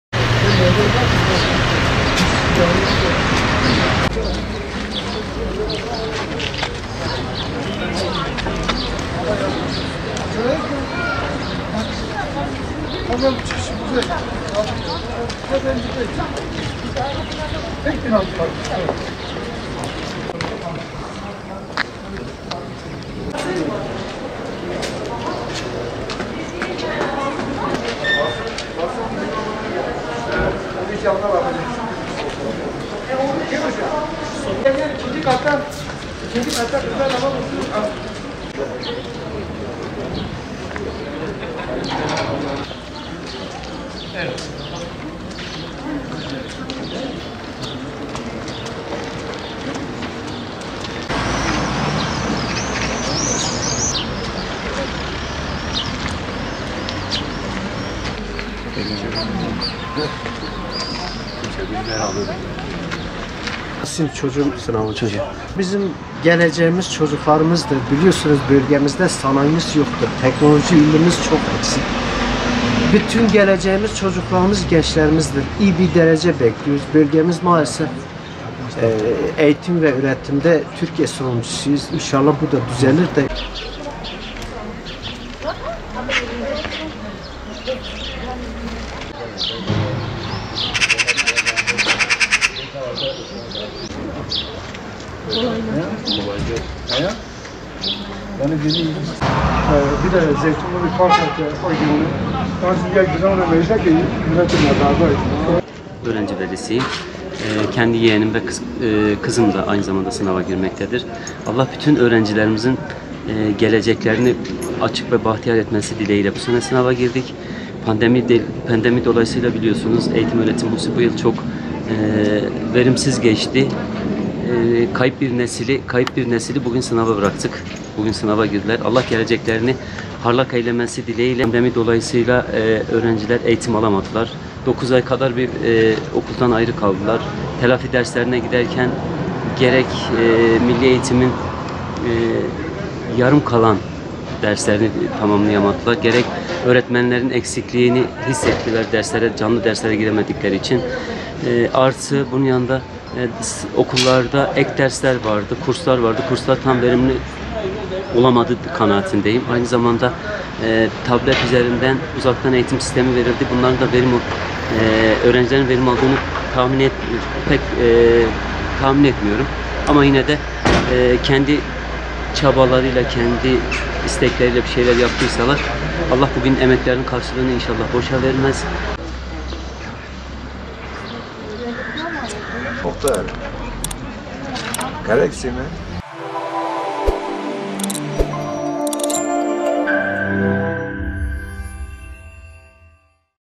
Ağrı'da YKS'ye Giren Öğrencilerin Aileleri Çocukları İçin Dua Etti
Cumartesi ve Pazar gerçekleşecek Yüksek Öğretim Kurumları Sınavı (YKS) için öğrenciler sınıflarda ter dökerken aileler ise öğrencilerin başarılı olabilmesi için dua ettiler.